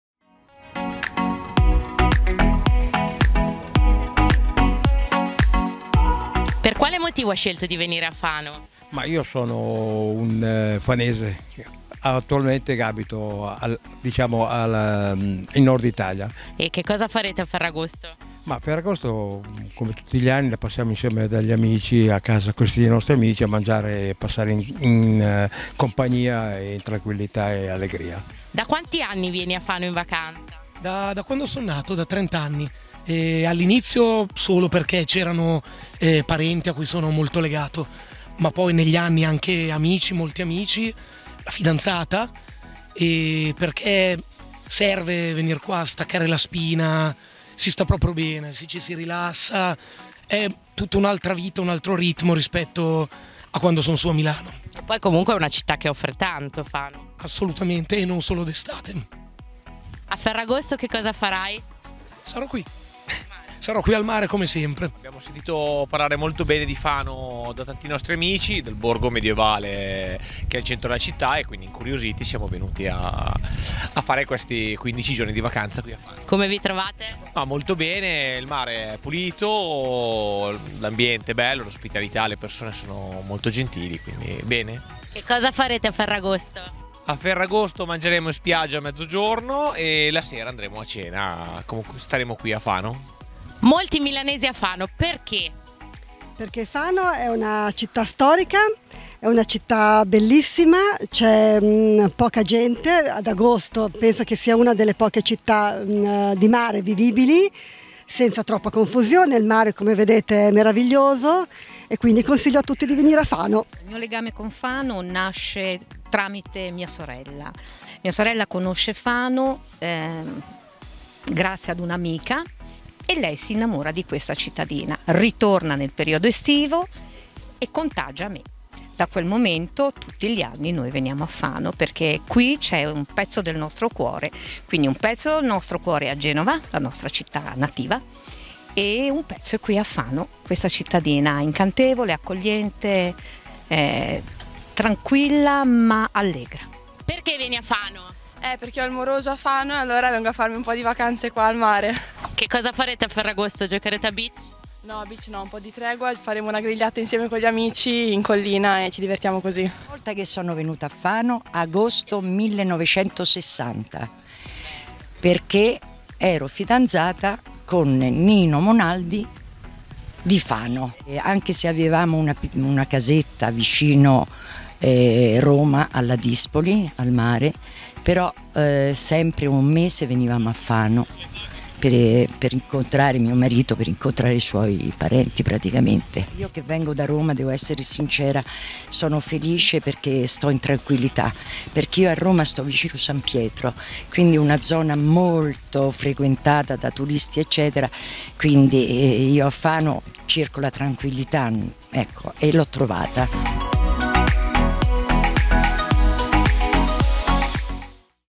Vacanza a Fano: le interviste ai turisti
Fano è meta di tanti turisti a cui abbiamo chiesto per quale motivo scelgono la città della Fortuna come destinazione per le loro vacanze.